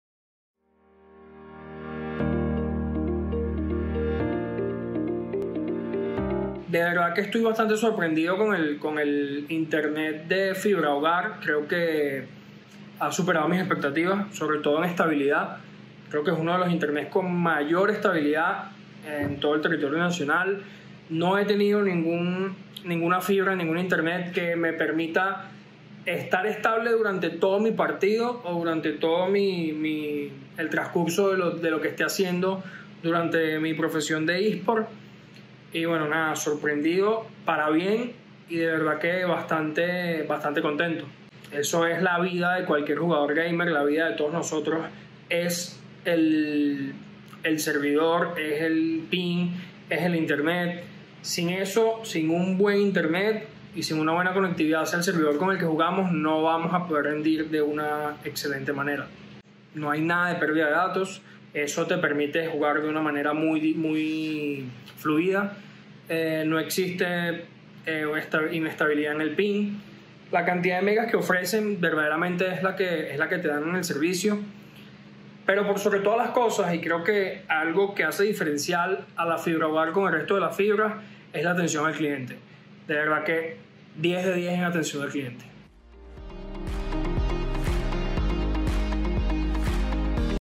Fibrahogar es la opción para la comunidad gamer
Gana sin interrupciones ni lag en deportes electrónicos y juegos online!

Actualizamos la red y te llevamos nueva tecnología con fibra óptica directa a casa para que disfrutes una conexión que se mueve a la velocidad de la luz, sin interferencias, y con interacción insuperable entre el ritmo del juego y la realidad.

o escríbenos al WhatsApp 0412-00INTER (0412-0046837) y pasa al siguiente nivel, como José Kazanji con el Esports Caracas Fútbol Club.